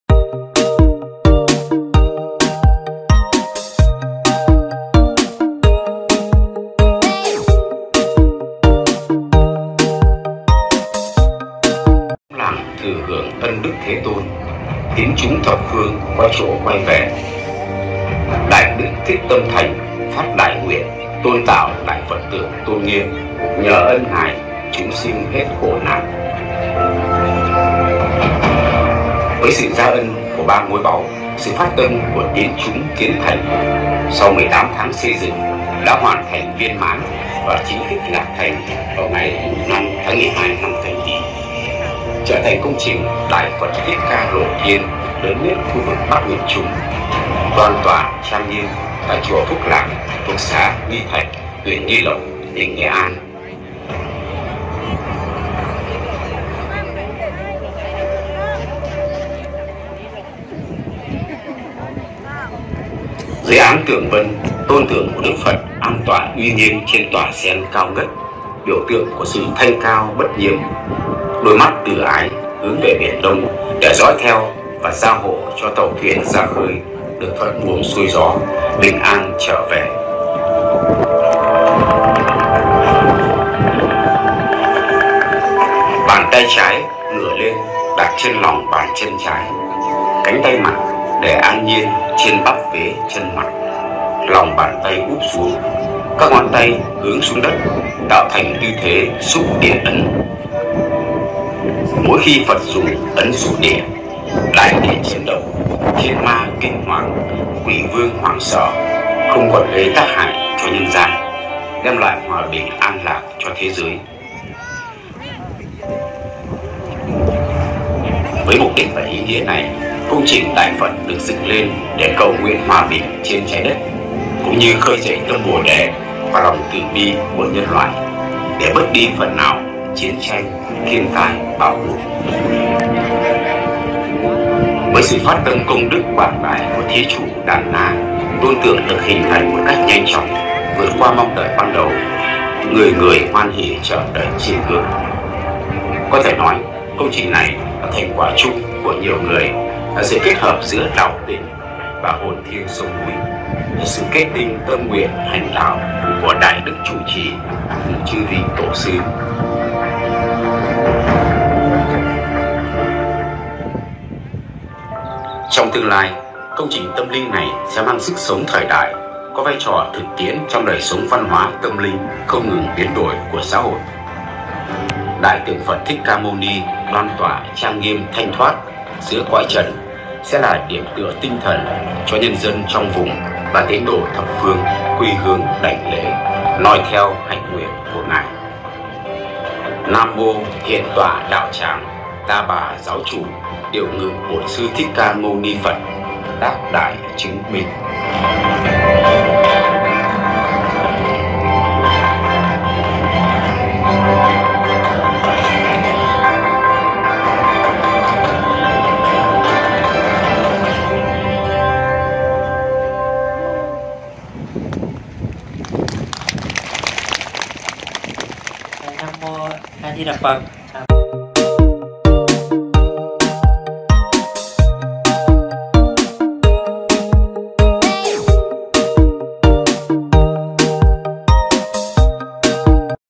Ý Nghĩa Của Đại Tượng Phật Thích Ca Mâu Ni 42 Mét Tại Chùa Phúc Lạc
Hiểu hơn một chút về ý nghĩa của đại tượng Phật Thích Ca Mâu Ni cao 42 mét tại Chùa Phúc Lạc, Nghi Thạch, Nghi Lộc, Nghệ An. Đây là tượng Phật lộ thiên cao nhất Nghệ An cũng như Bắc Trung Bộ.